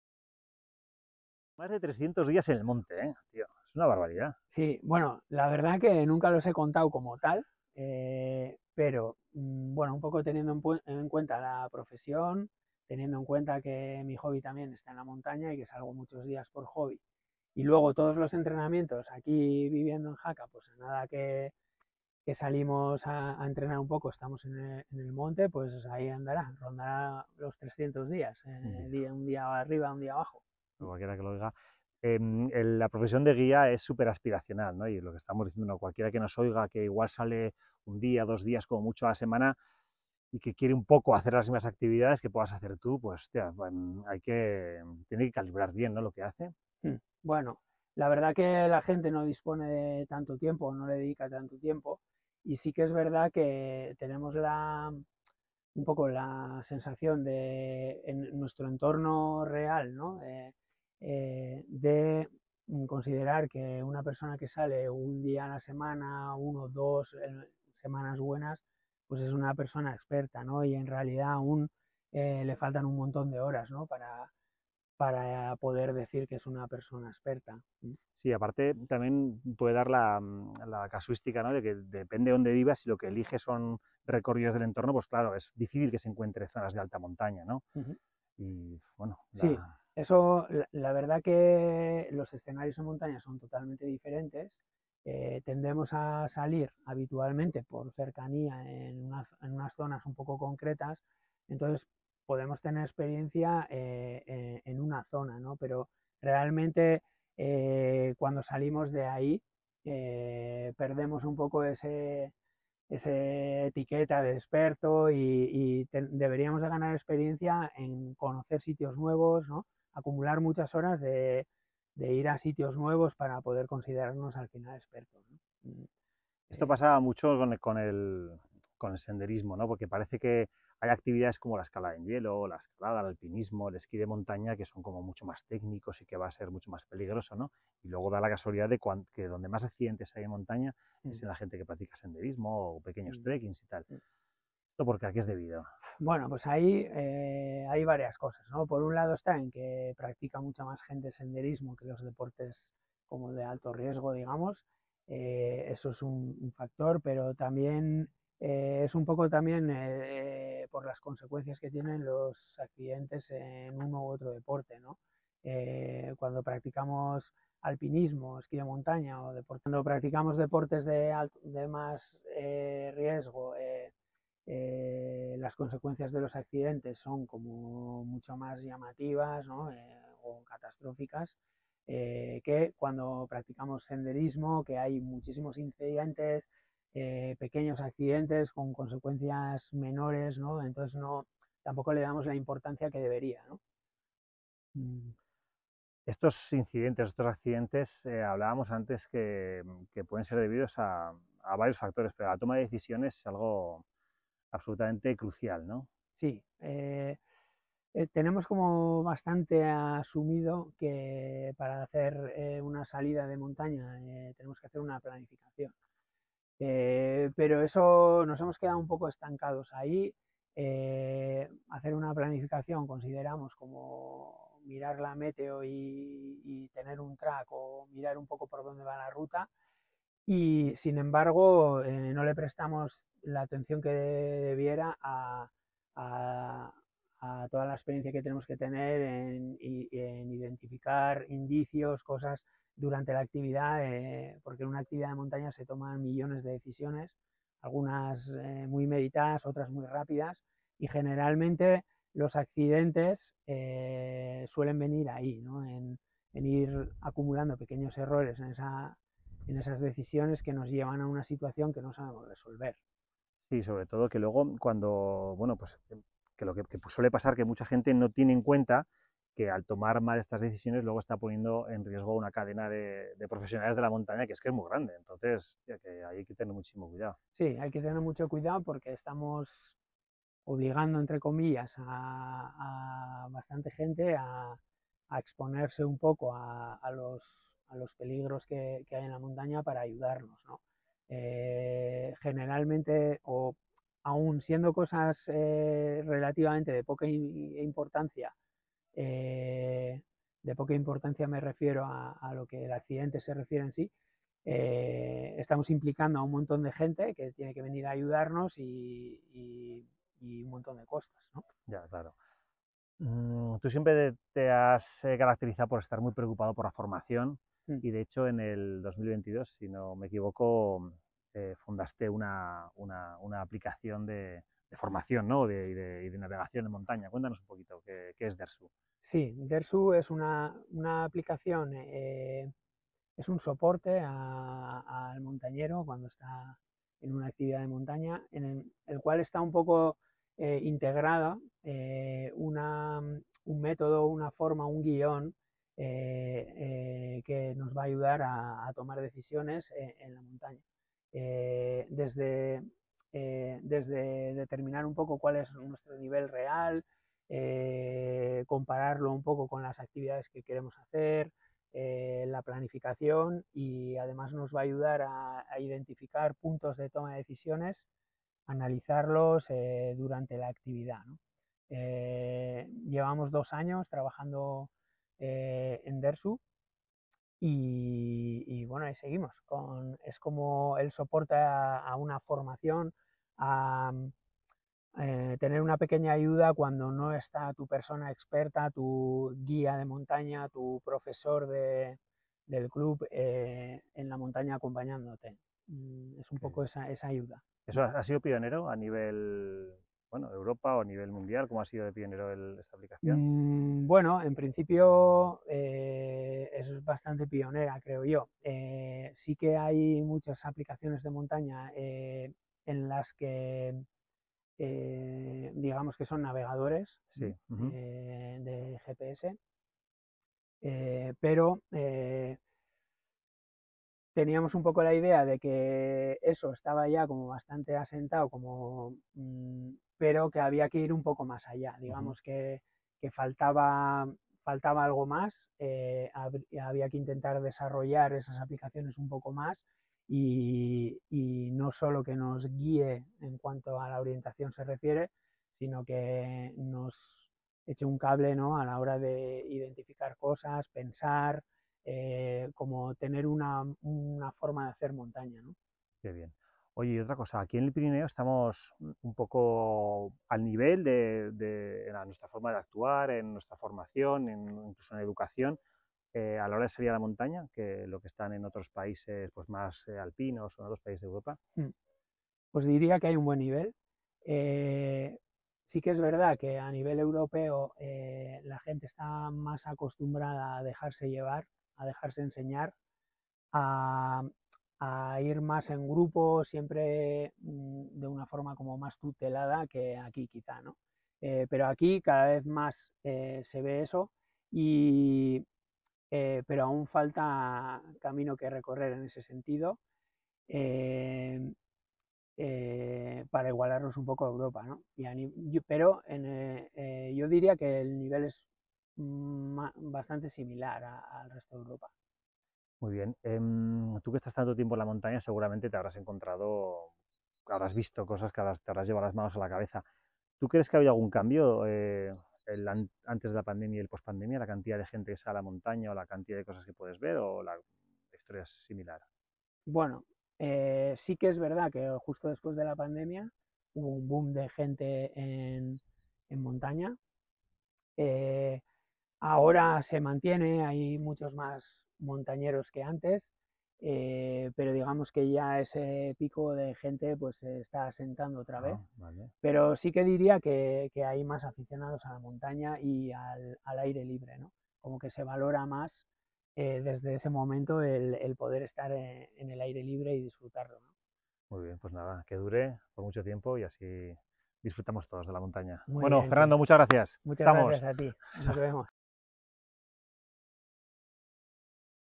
Entrevista Fernando Errekalde guía de alta montaña UIAGM,
🎥🌄 En este video, charlamos con Fernando Errekalde, guía de alta montaña UIAGM, para hablar sobre su vasta experiencia en montañismo, la importancia de los guías, la seguridad en la montaña y su  proyecto: la app Dersu.

Fernando nos comparte sus conocimientos y nos explica cómo la app Dersu puede transformar nuestra manera de planificar y tomar decisiones en las travesías y actividades de montaña. No te pierdas esta interesante charla llena de consejos y anécdotas de uno de los expertos más reconocidos en el mundo del montañismo.

🔗 ¡Dale play y descubre más sobre el fascinante mundo de la montaña con Fernando Errekalde!